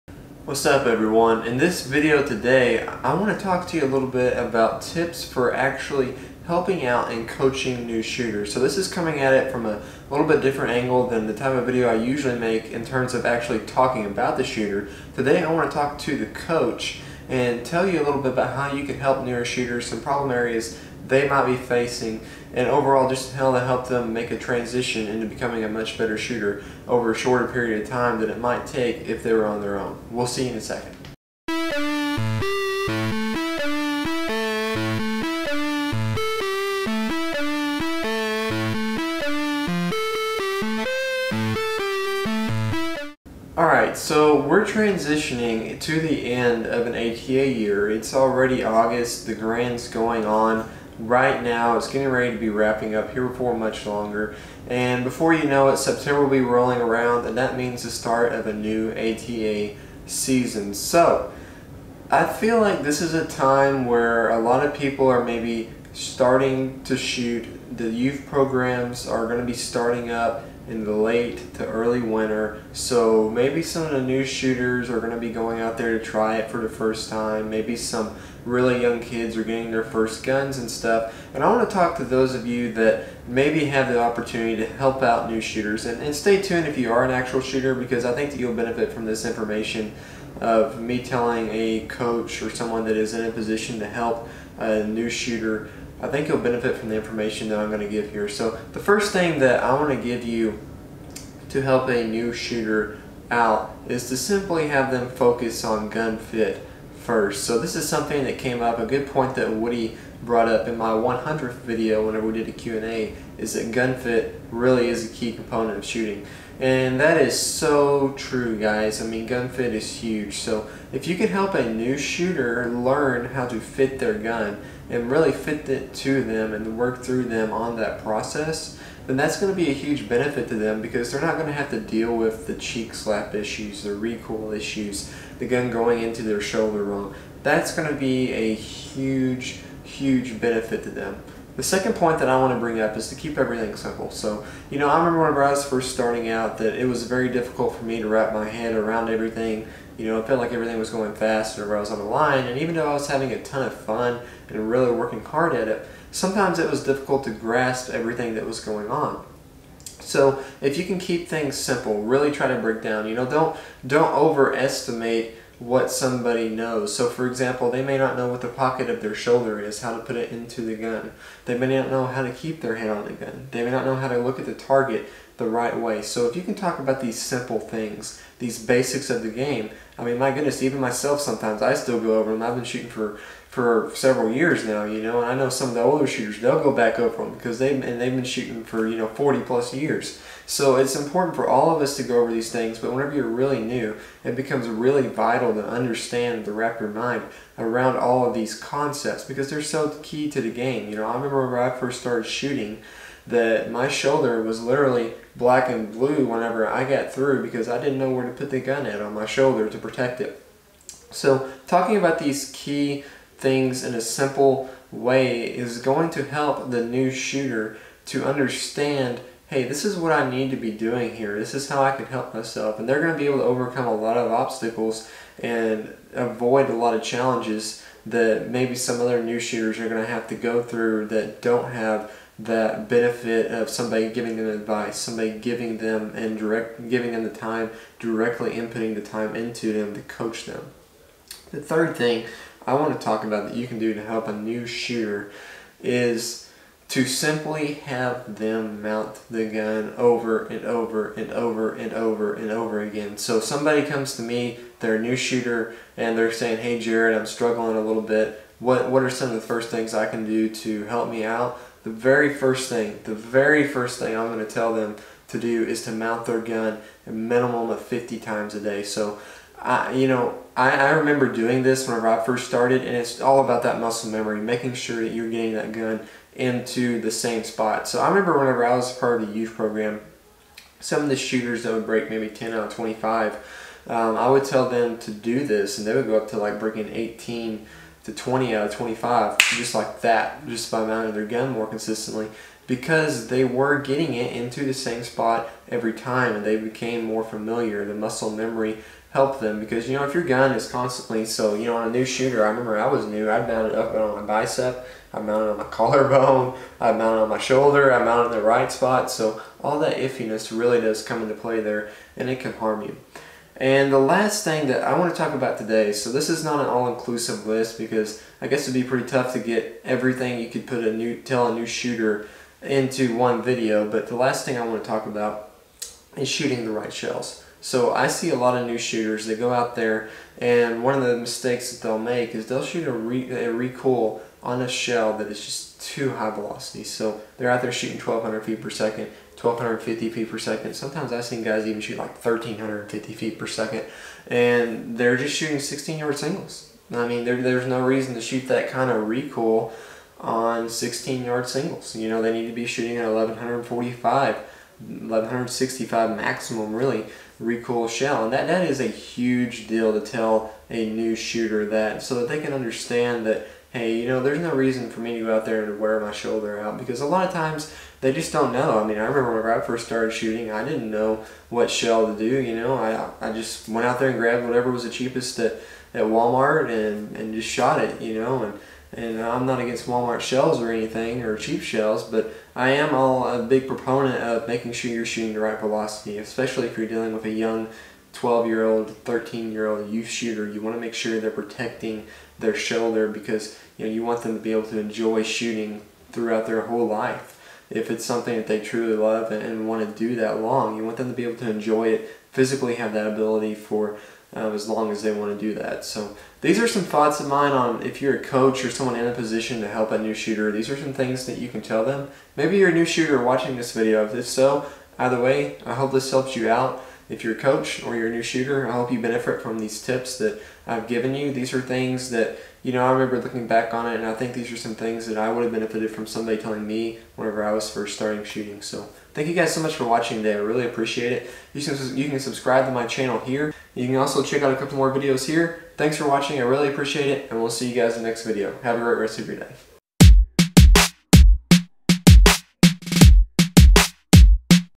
Tips for helping new shooters!!!
In this video, I am going to give you a few tips you can use to help out new shooters.